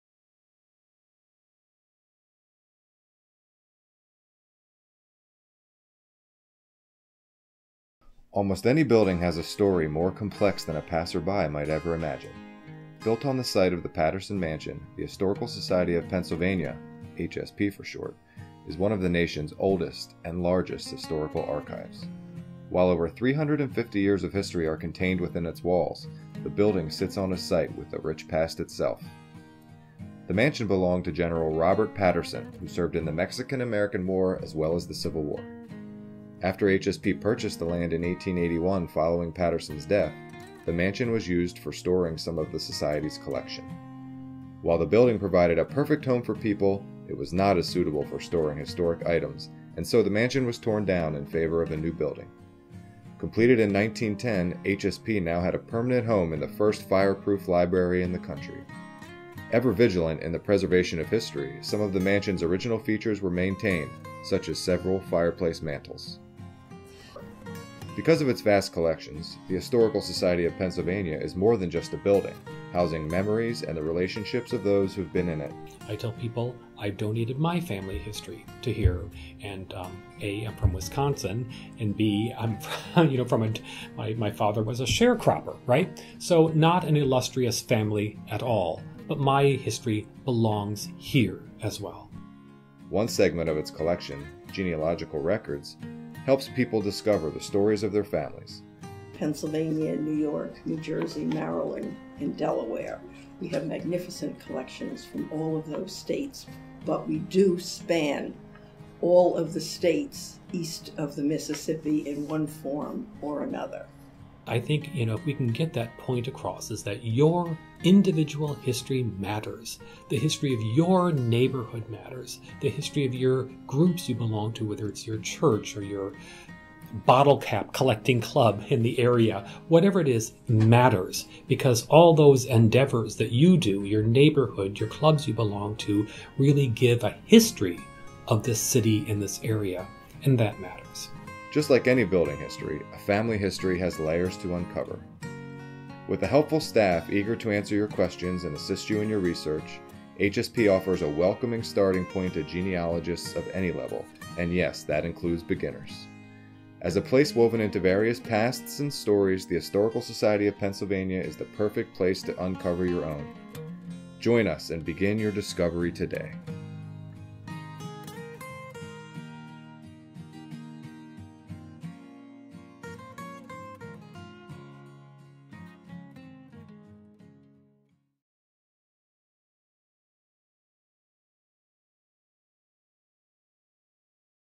Make History Yours at HSP
Everything, and everyone has a story. From buildings to neighborhoods to people - each individual story matters. Do you know yours? Find it at HSP.

This video was created by Gina Reitenauer, Digital Video intern.